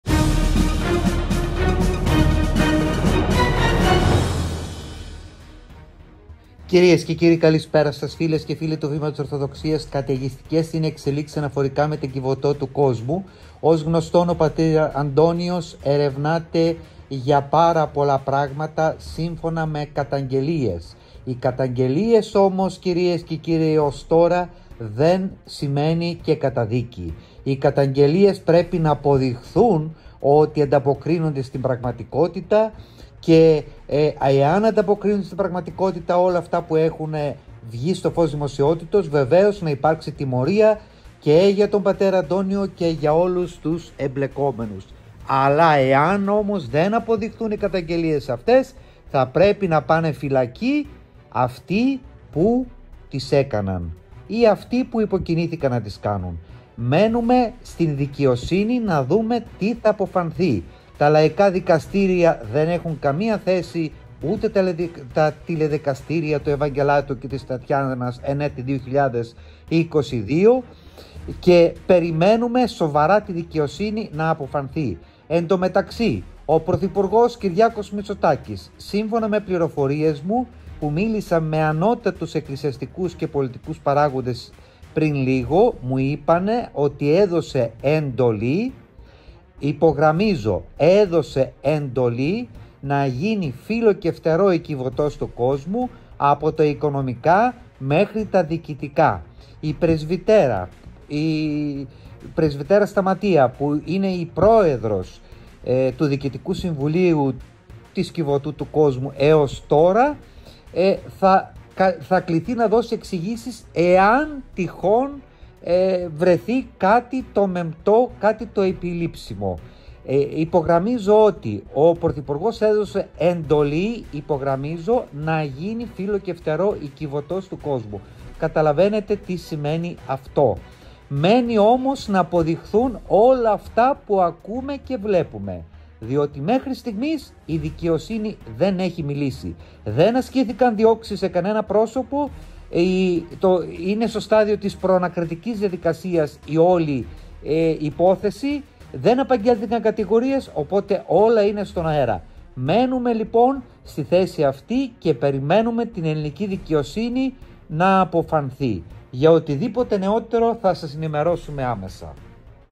ΕΝΤΟΛΗ ΜΗΤΣΟΤΑΚΗ για Πατήρ Αντώνιο, Πρεσβυτέρα και ΚΙΒΩΤΟ ΤΟΥ ΚΟΣΜΟΥ! Εδόθη πριν λίγο!
ΠΑΤΗΡ ΑΝΤΩΝΙΟΣ ΚΑΙ ΜΗΤΣΟΤΑΚΗΣ!!

Ραγδαίες εξελίξεις για την ΚΙΒΩΤΟ ΤΟΥ ΚΟΣΜΟΥ. Ο Κυριάκος Μητσοτάκης έδωσε εντολή πριν λίγο να κάνουν φύλλο και φτερό τα οικονομικά και ότι αφορά την λειτουργία της ΜΚΟ!

ΡΕΠΟΡΤΑΖ - Γιώργος Θεοχάρης - ΒΗΜΑ ΟΡΘΟΔΟΞΙΑΣ